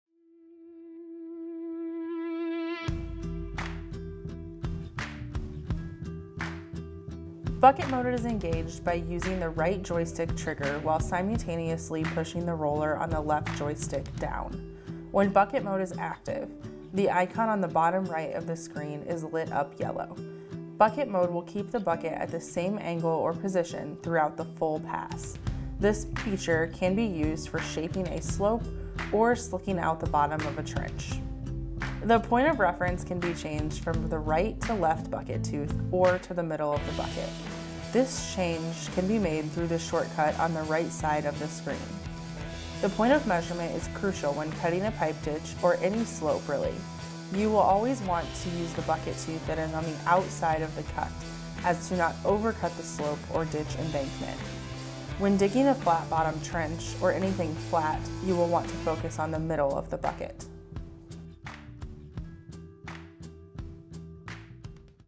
How to Engage Bucket Mode | John Deere SmartGrade™ Excavators
How to engage and use bucket mode when using SmartGrade Integrated Grade Control on John Deere excavators.